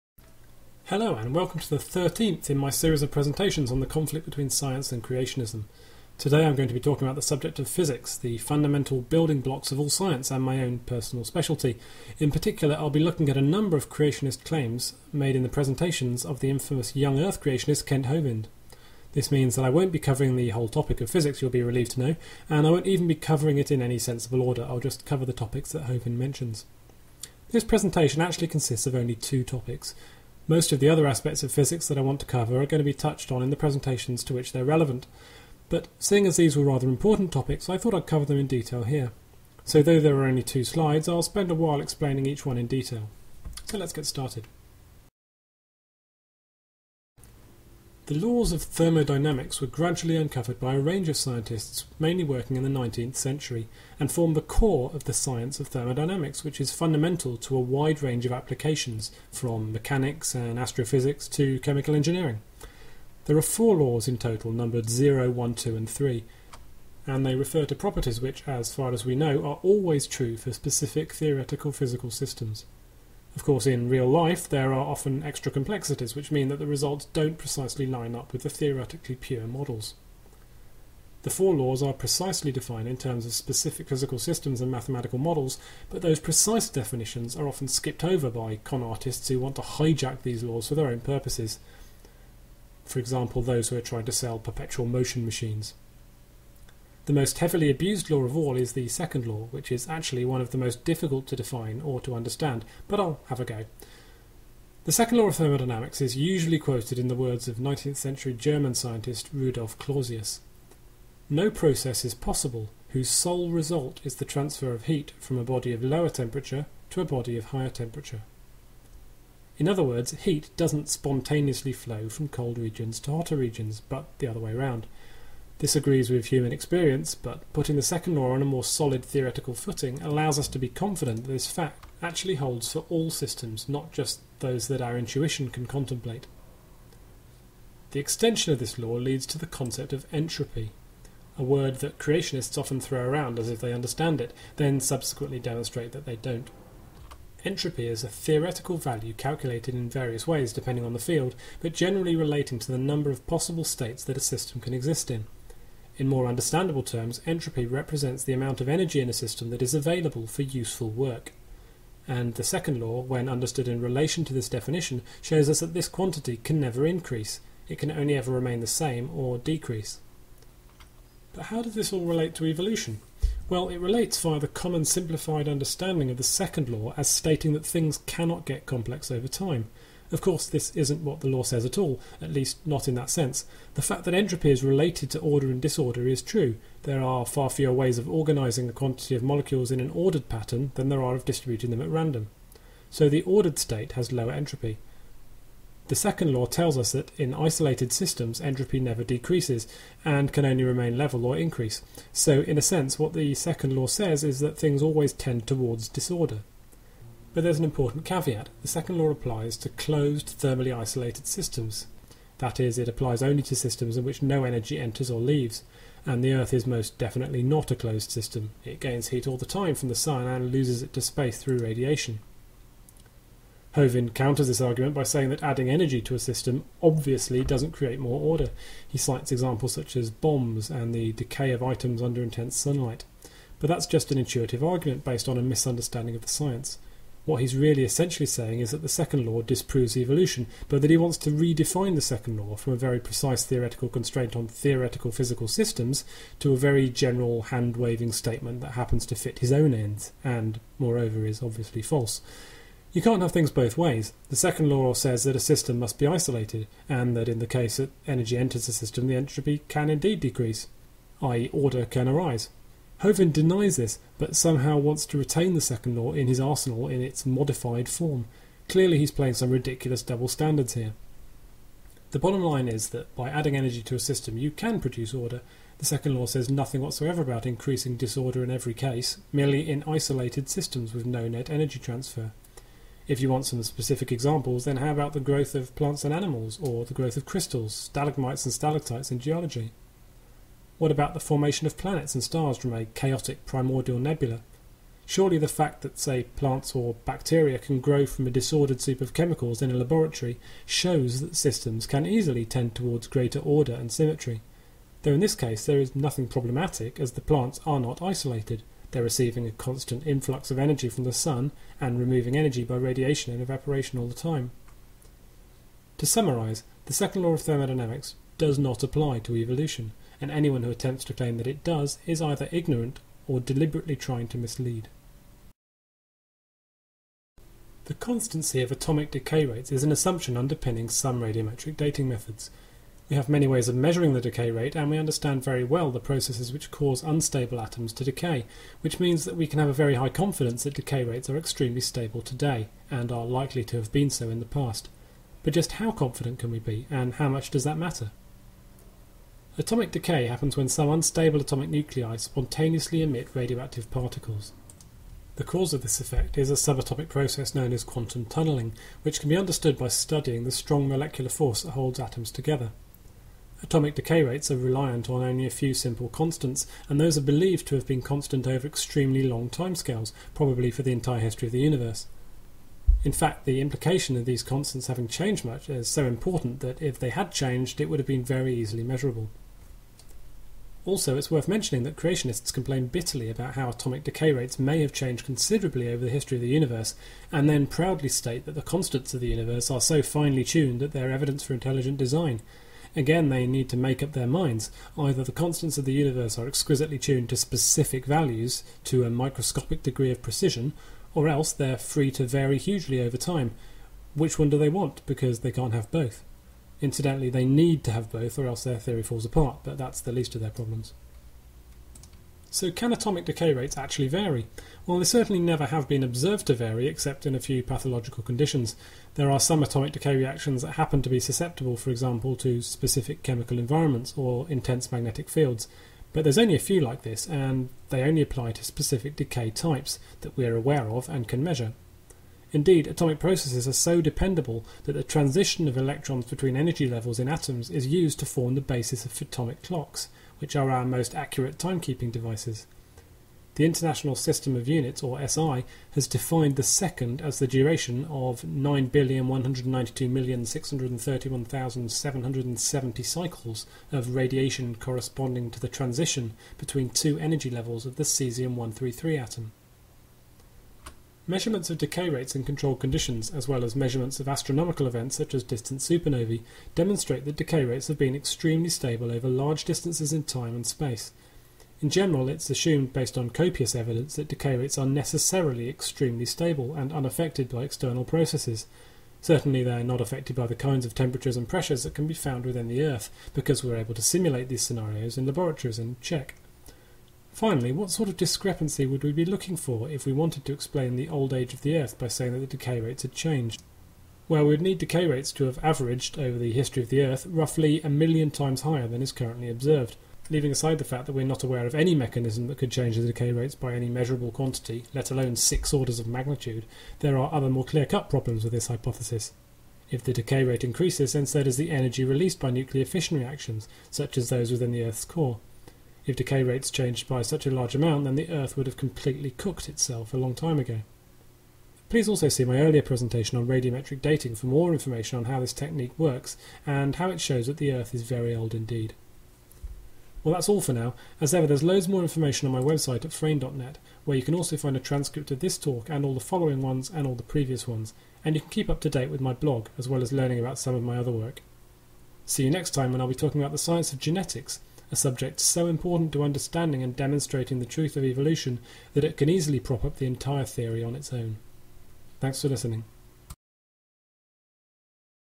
Presentation 13 : Physics
This is the thirteenth part of my lecture series on Science and Creationism. Today I'm looking at the subject of Physics, and approaching the two topics of Entropy and Atomic Decay Rates in more detail.